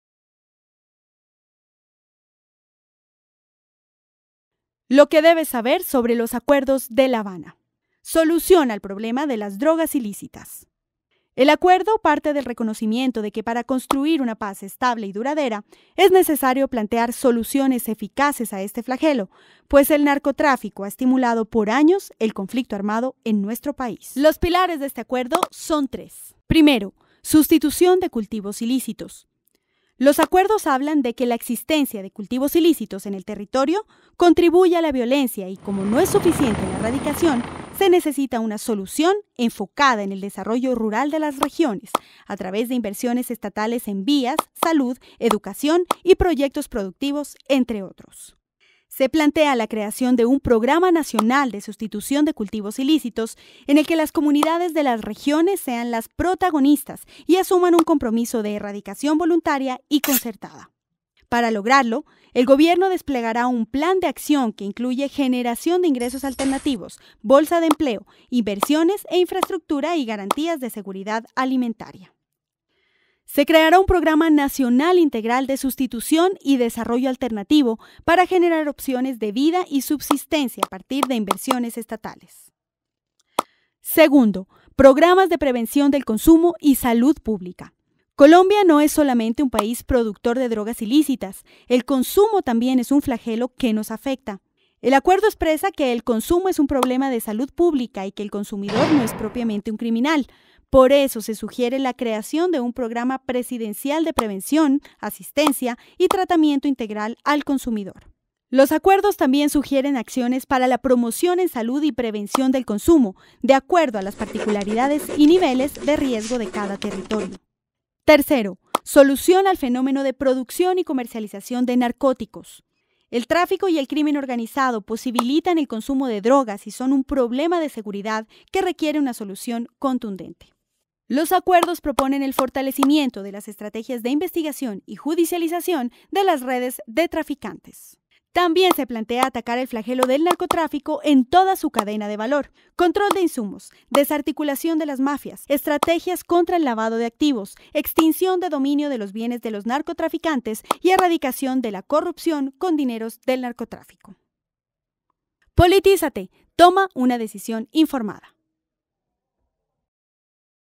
Acuerdo #4 – Solución al problema de drogas ilícitas
Seguimos promoviendo el voto informado, es por eso que hoy te contamos todo sobre el punto No. 4 de los acuerdos de paz, para que el próximo 2 de octubre puedas tomar una decisión a conciencia.